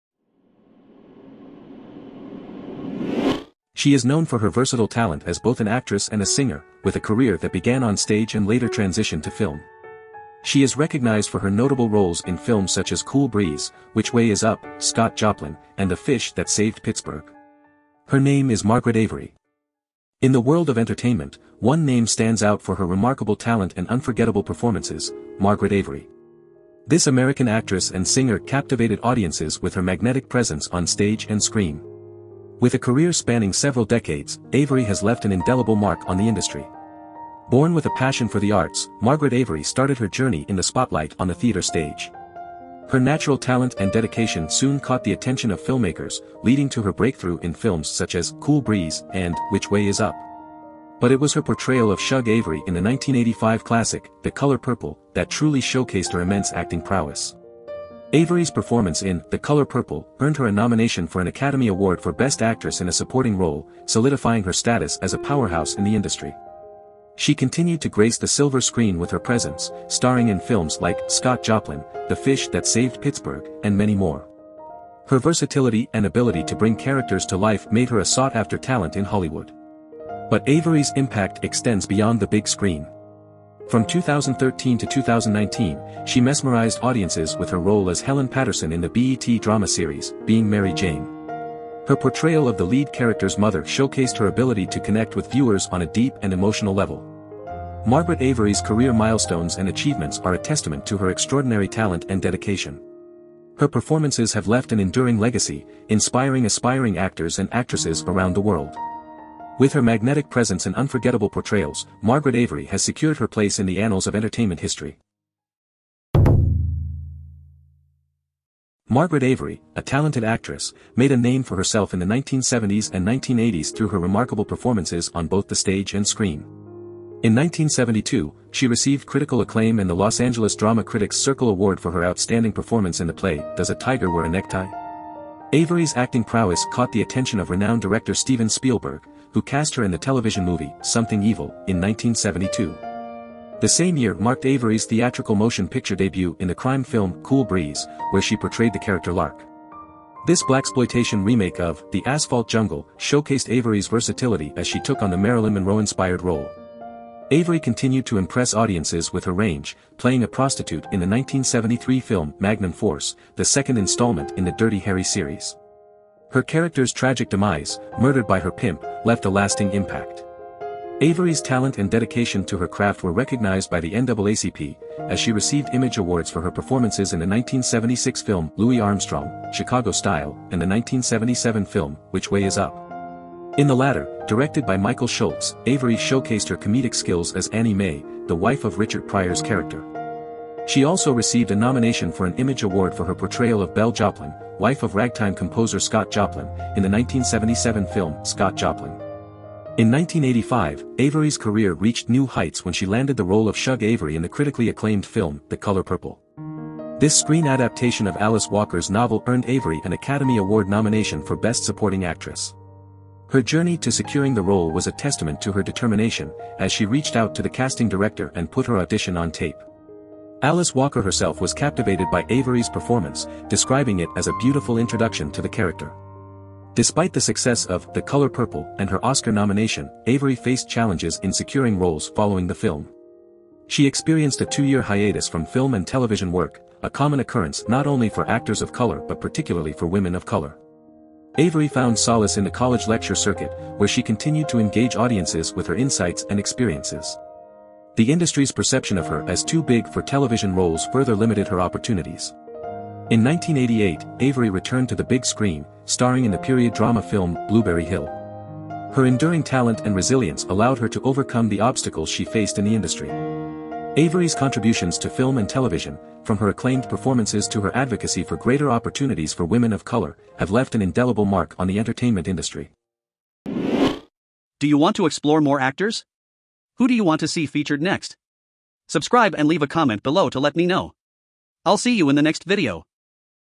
Margaret Avery: From Stage to Screen | Actors & Actresses Biography
Among the plays Avery appeared in were Revolution and The Sistuhs. In 1972, she received the Los Angeles Drama Critics Circle Award for Outstanding Performance…

854e